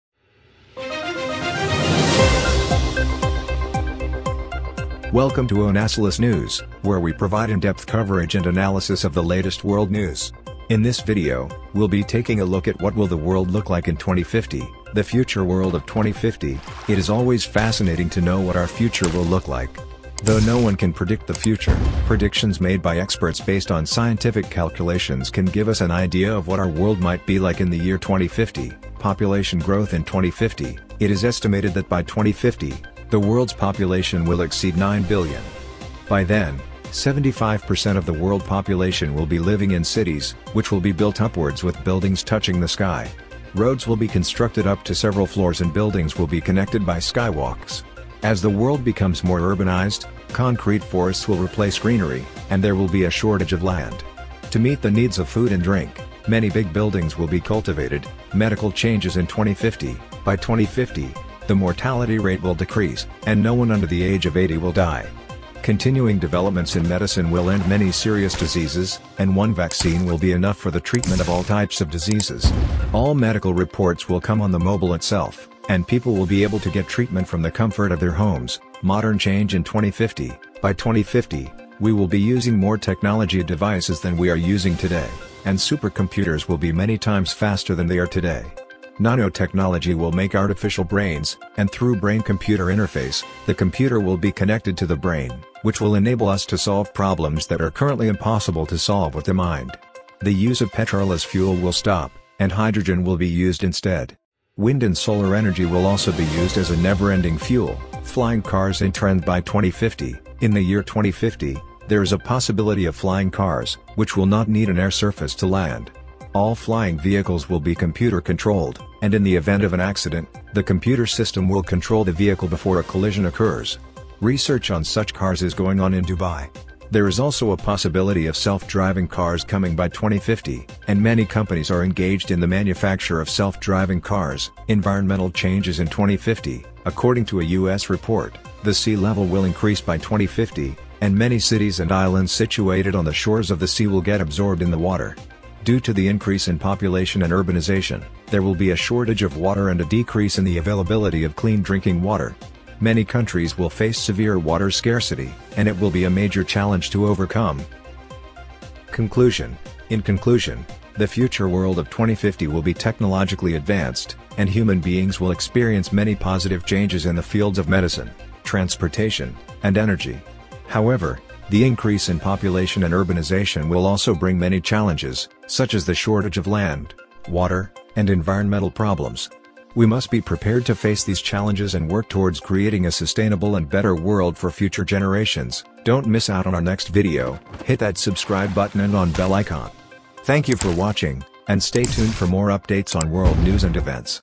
What will the world look like in 2050?
Onasilus News brings you world news.

The Future World of 2050

It is always fascinating to know what our future will look like. Though no one can predict the future, predictions made by experts based on scientific calculations can give us an idea of what our world might be like in the year 2050.